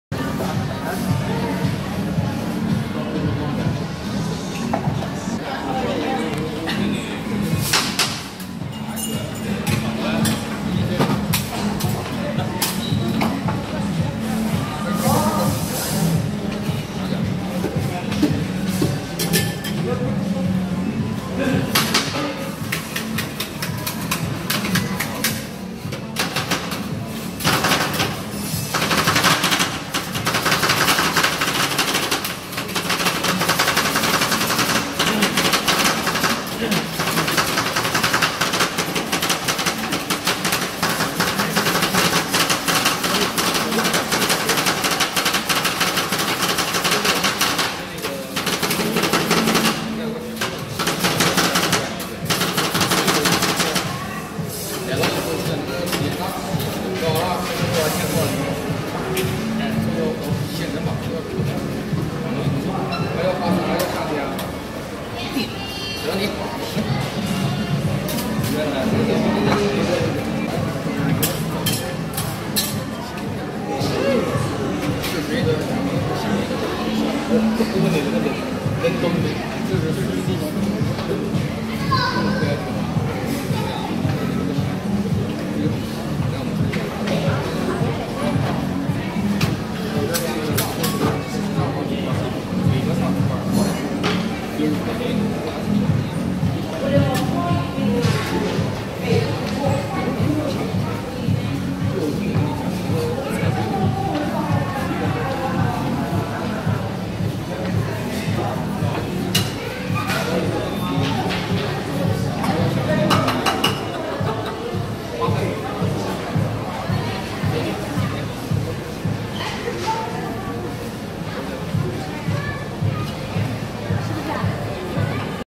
BALI INDONESIA ICE CREAM CHEF | TRAVEL VLOG PART 1
Watch this cool vlog in Bali where this talented chef creates a delicious ice cream treat. I hope you enjoyed watching this video.